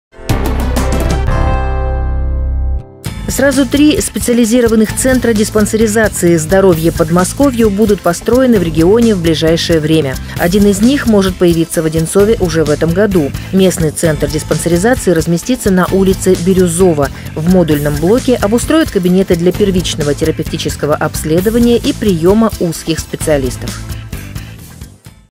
Центр диспансеризации появится в г.Одинцово
Сразу три специализированных центра диспансеризации «Здоровье Подмосковью» будут построены в регионе в ближайшее время. Один из них может появиться в Одинцове уже в этом году.